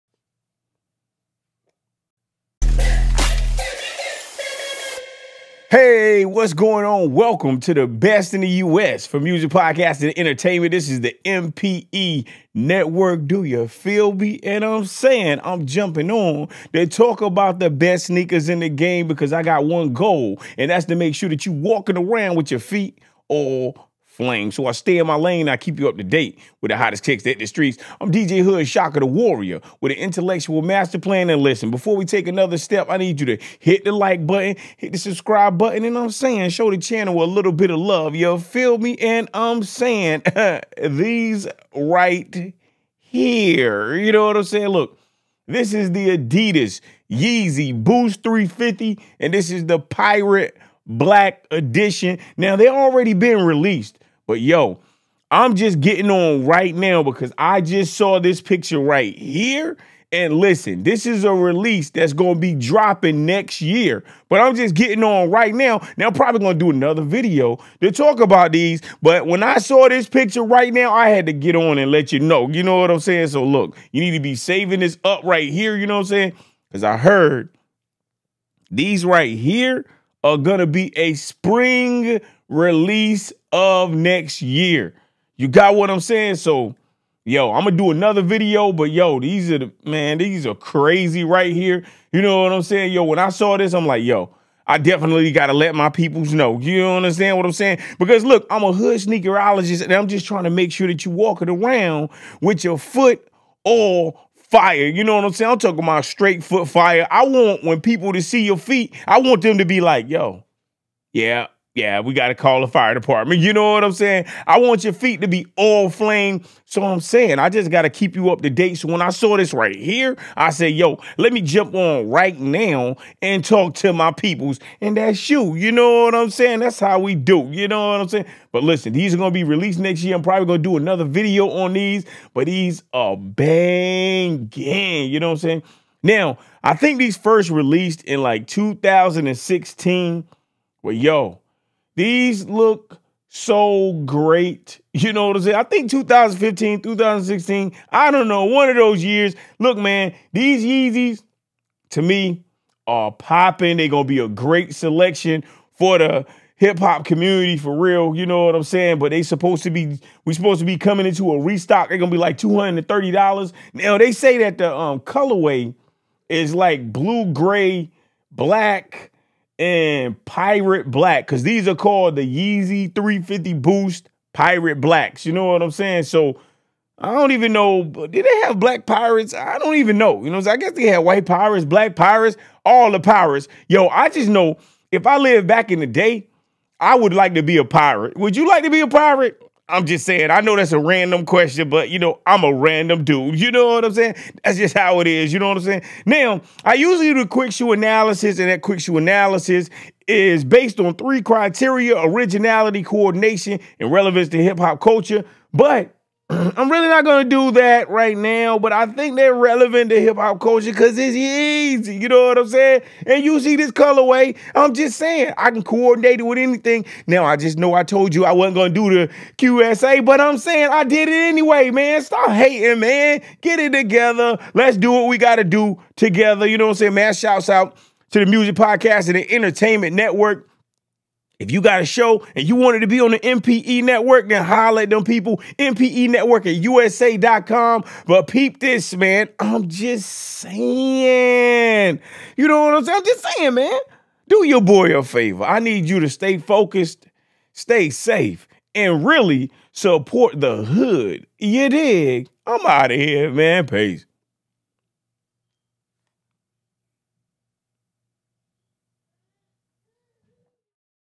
The Adidas Yeezy Boost 350 Pirate Black
In This Episode Of Wish Me Luck . We Are Talking About The The Adidas Yeezy Boost 350 Pirate Black. The Real Question Is Are You Coppin Or Do You Think They Are Floppin?